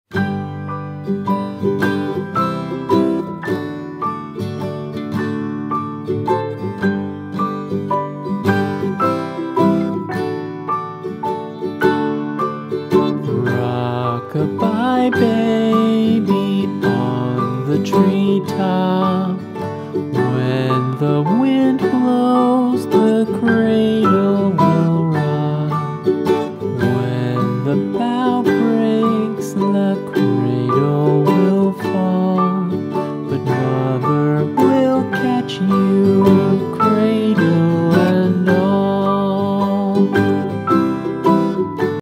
bentwood rocker rebuild
repaired and old bentwood rocker for a child